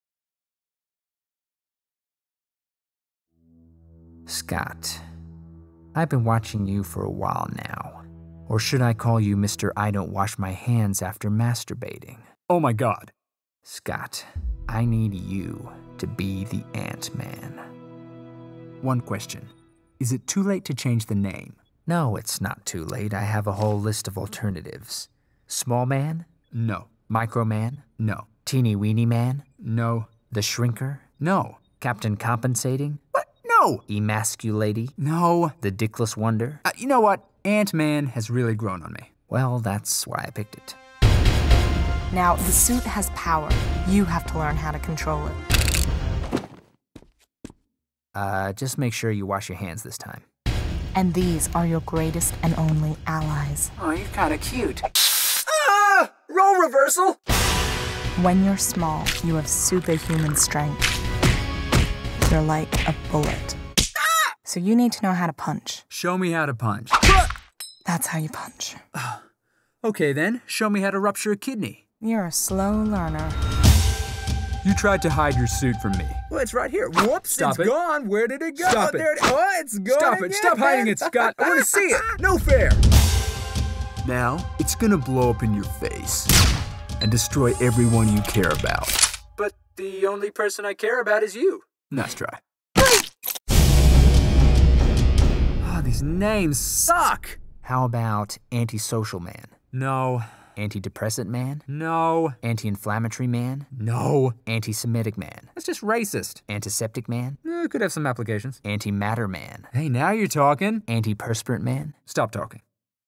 Ant-Man Trailer Spoof - TOON SANDWICH
An animated parody of the Ant-Man trailer, in which Paul Rudd experiences significant shrinkage, Evangeline Lilly demonstrates how to sucker-punch and Michael Douglas indulges a basic instinct. Put on your Yellow Jacket cos it's about to get Rudd-iculous!

HIDDEN BREADCRUMBS
Can you find the 5 hidden references to the other Seasoning 1 spoofs in this video?